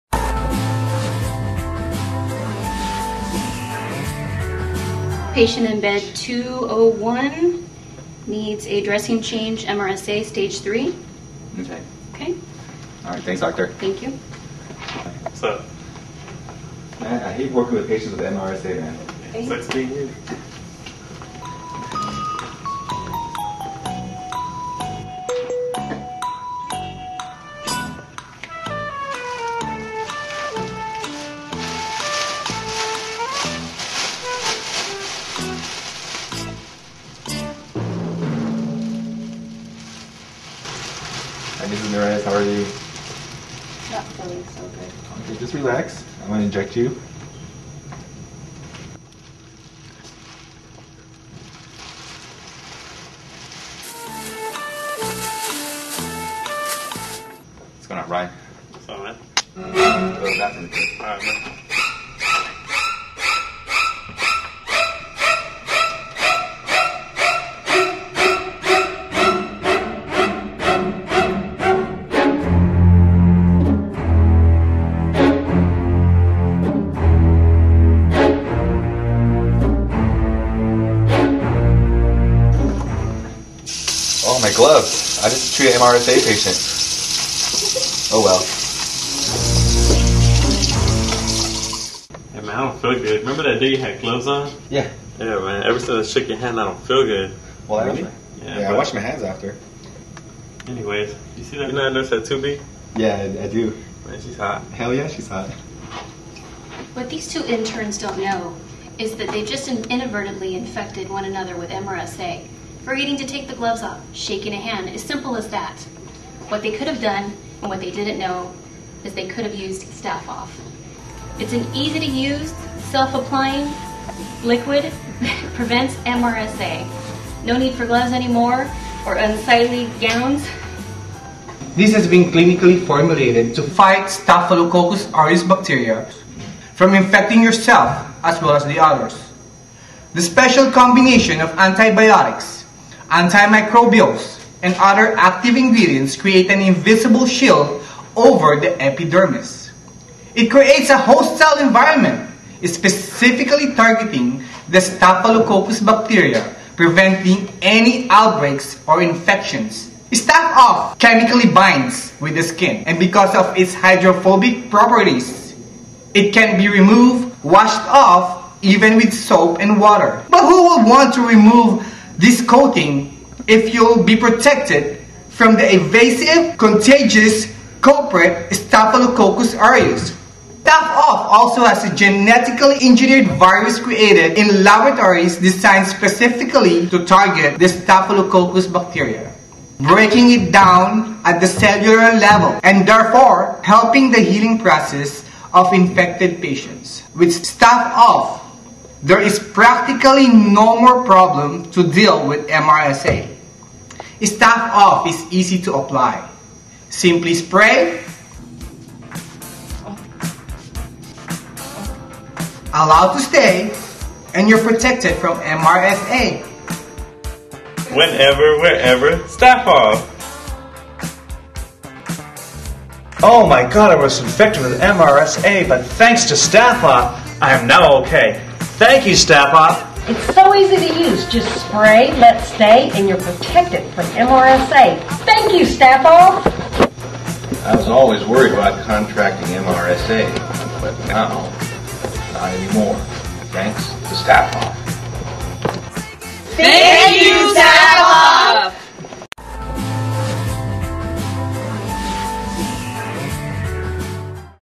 Disease Project
The students at Concorde Career College Garden Grove put this video together for a project in which they were to invent a new cure for a current disease.  The class is taught by Mr. Joel Boyd, RCP RRT. Special thanks to the education department administration staff at Concorde Career College, Garden Grove for their participation. Cast includes Daisy Ramirez, Laurie McClymonds, Brian Herrera, Kalvin Ng, & Rhandy Torres. Directed and Edited by Raleigh Araga.